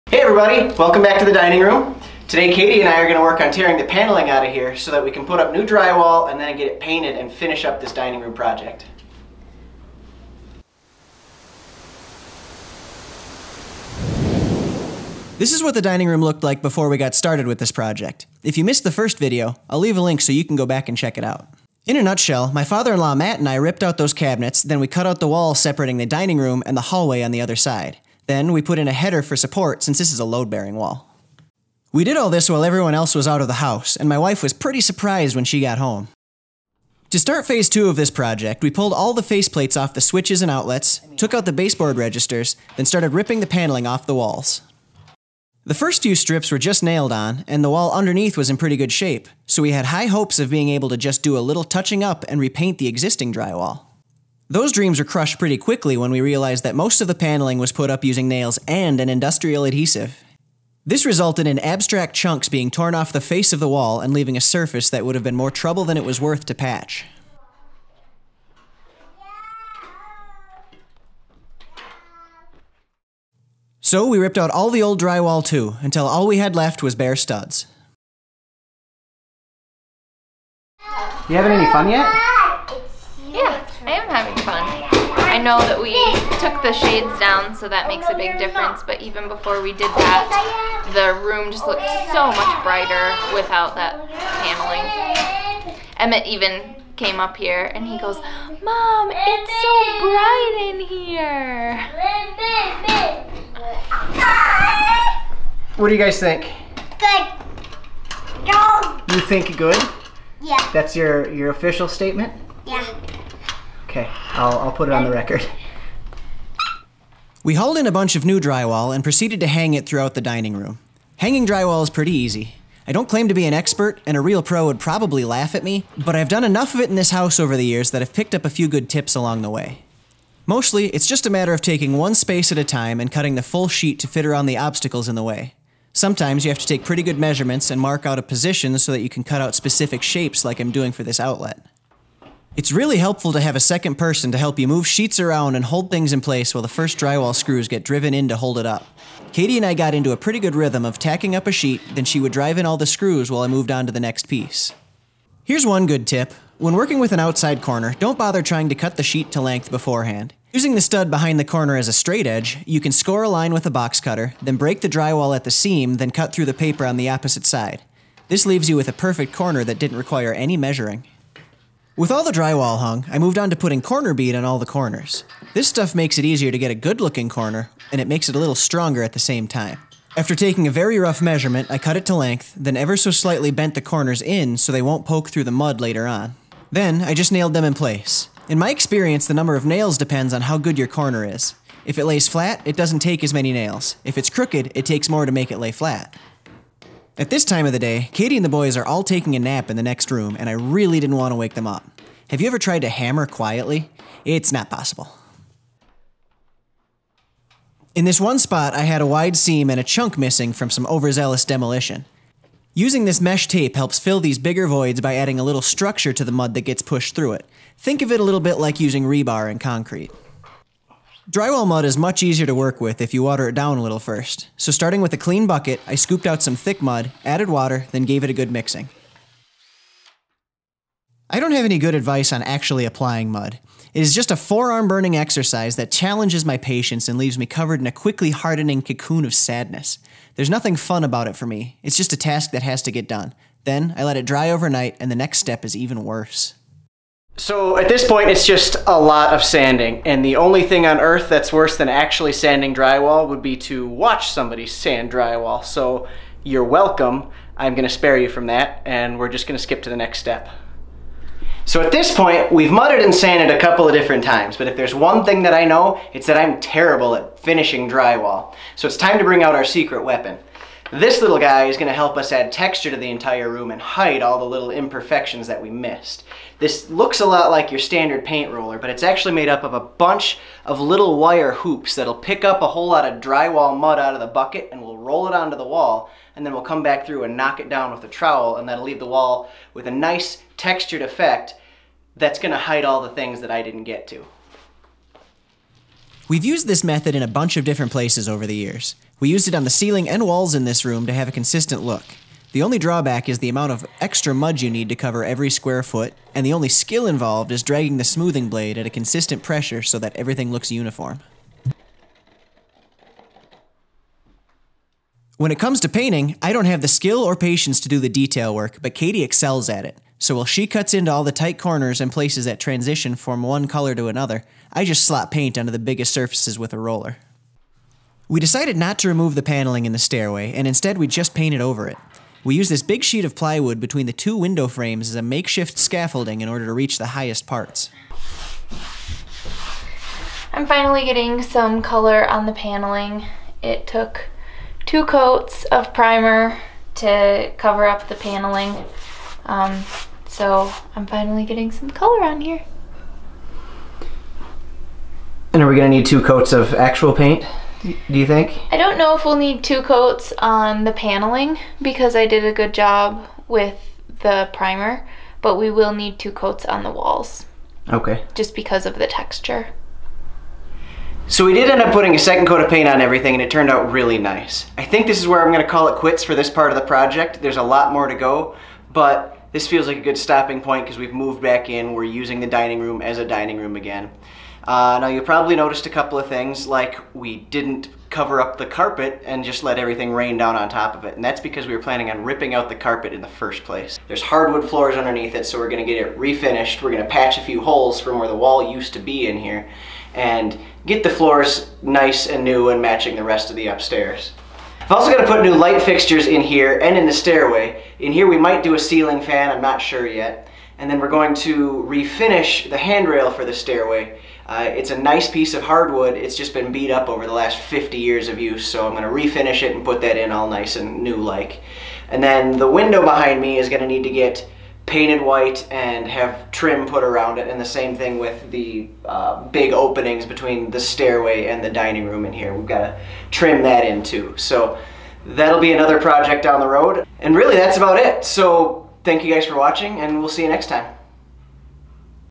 Updating The Dining Room
Time for another round of work on our dining room project. This time my wife and I rip out the beat up old paneling and pull down the old drywall. Then we hang all new drywall, mud, sand and paint the whole room and the stairway while we’re at it.

Gunflint Designs is a participant in the Amazon Services LLC Associates Program.